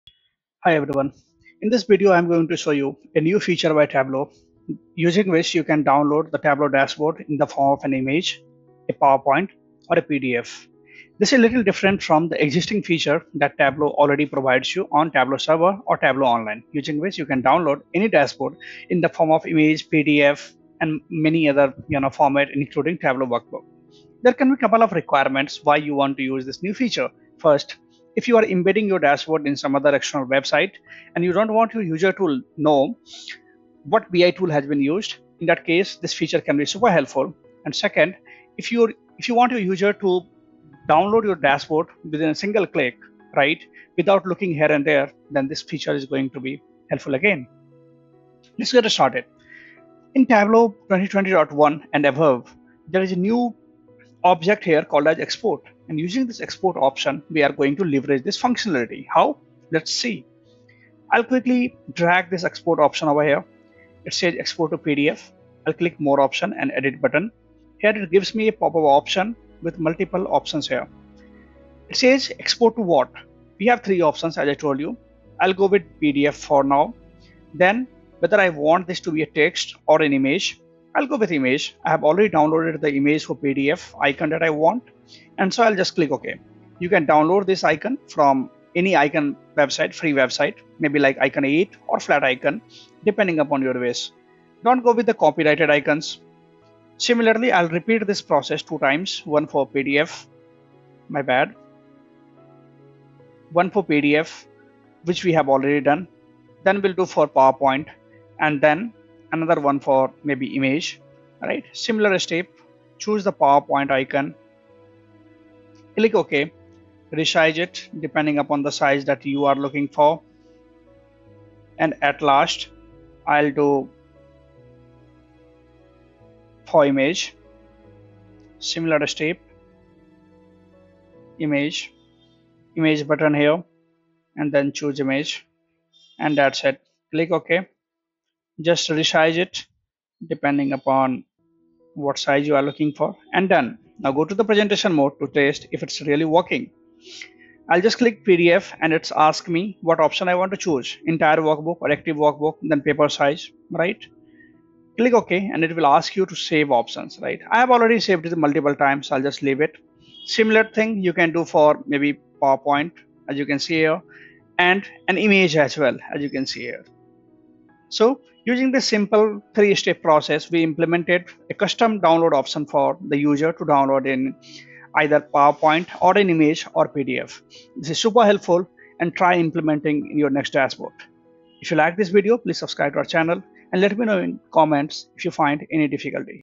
Add custom dashboard download button in Tableau
Learn how you can add custom download buttons for users to download your Tableau dashboard in the image, PDF, and PowerPoinr format easily.